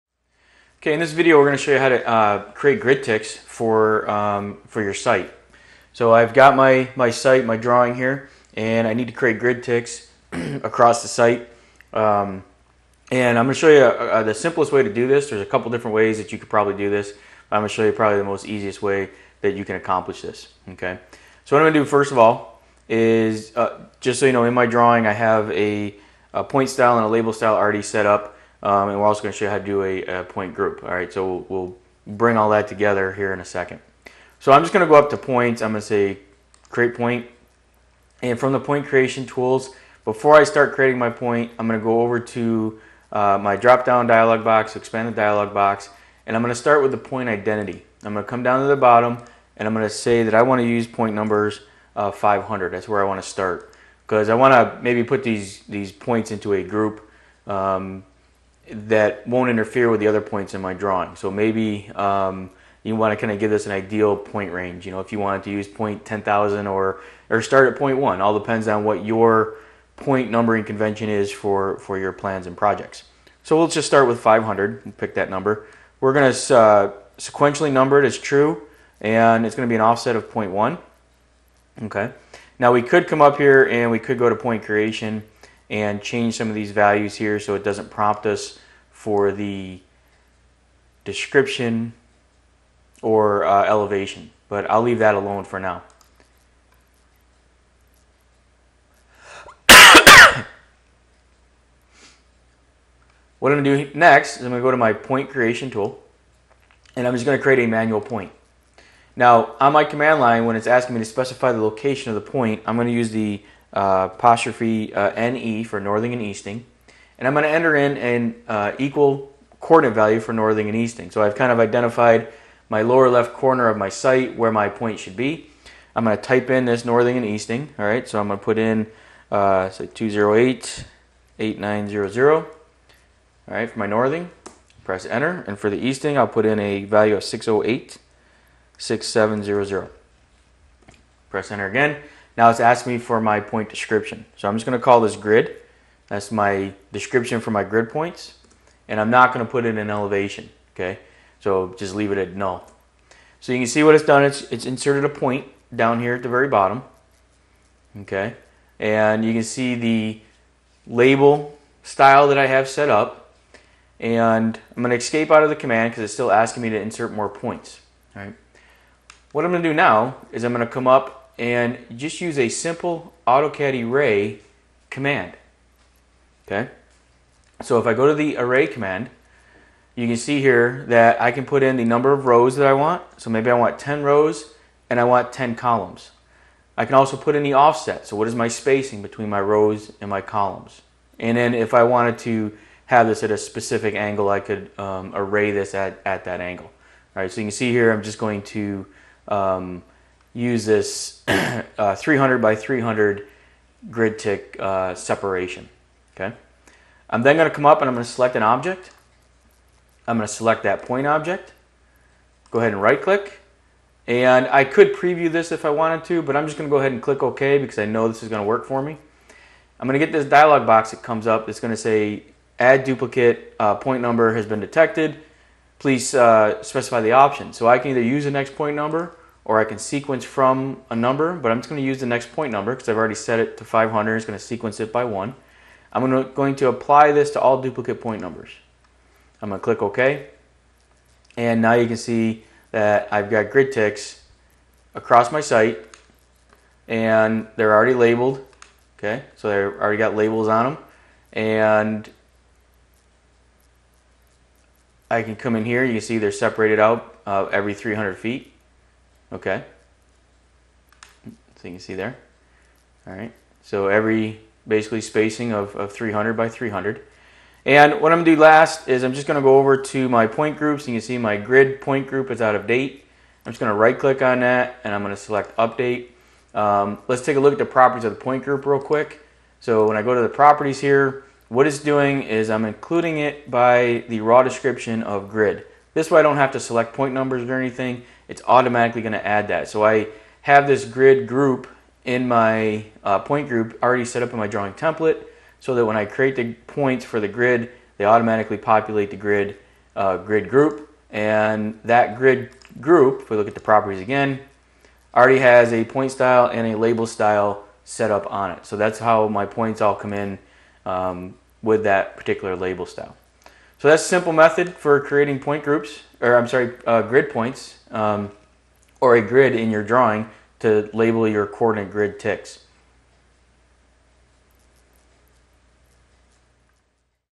Create Grid Ticks for a Map using AutoCAD Civil 3D
In this video we will demonstrate how to easily create Grid Ticks for a Map within Civil 3D.  More learning and tips can be found at theflyinglabs.com